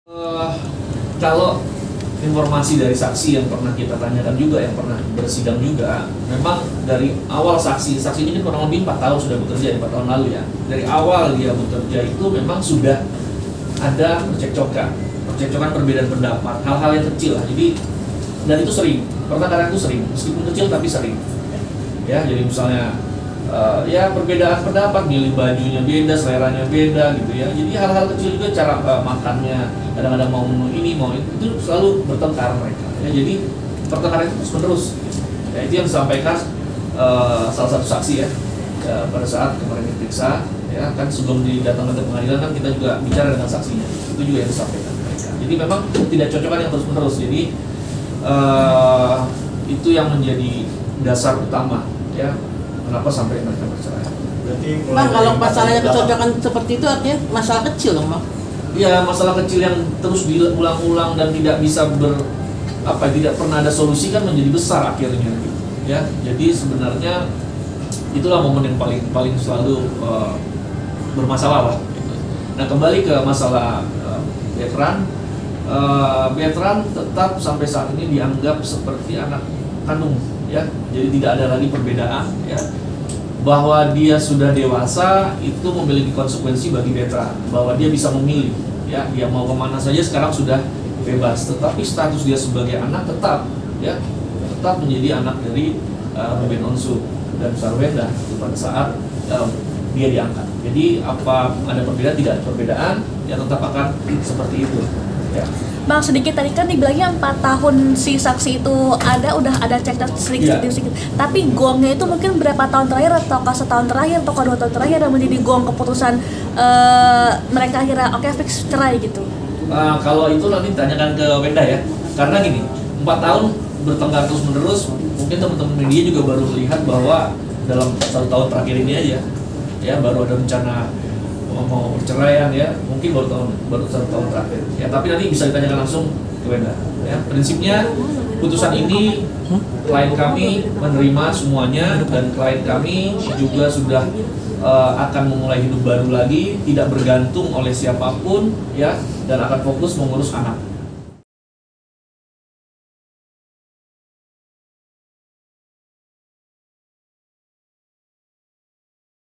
Baru Terungkap, Ruben Onsu dan Sarwendah Cekcok Sejak 4 Tahun Lalu
Ruben Onsu dan Sarwendah resmi bercerai pada Selasa (24/9/2024) di Pengadilan Negeri Jakarta Selatan. Soal alasan, pihak humas mengatakan sudah tidak ada kecocokan diantara keduanya.

Sementara itu, pihak Sarwendah yang diwakili pengacaranya mengatakan, cekcok menjadi alasan kliennya setuju bercerai dengan Ruben Onsu.